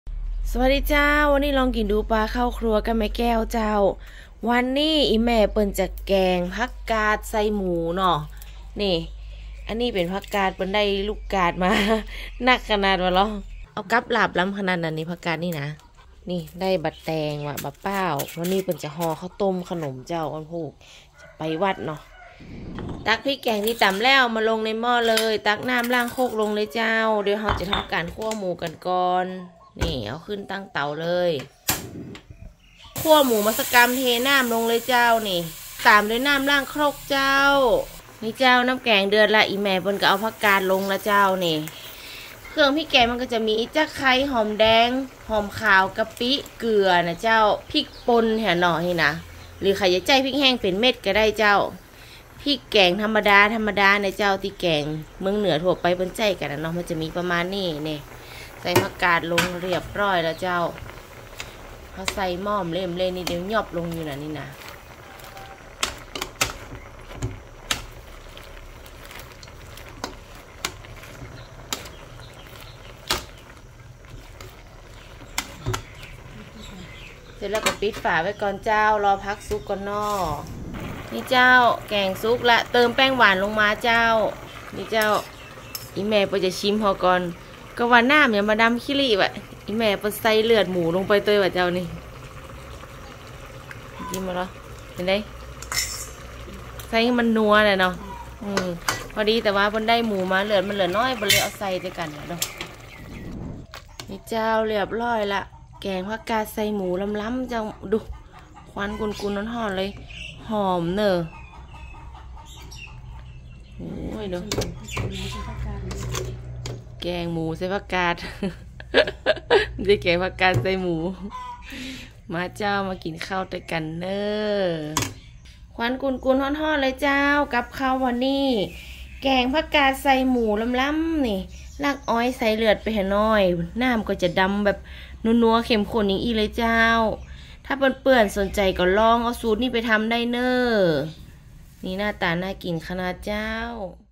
แกงผักกาดใส่หมู เลือดหมู รสเข้มข้นอร่อยมากๆ I ลองกินดู
วันนี้ลองกินดูพาทำ แกงผักกาดใส่หมู ใส่เลือดหมูลงไปด้วย รสชาตินัว เข้มข้น อร่อยมากจ้า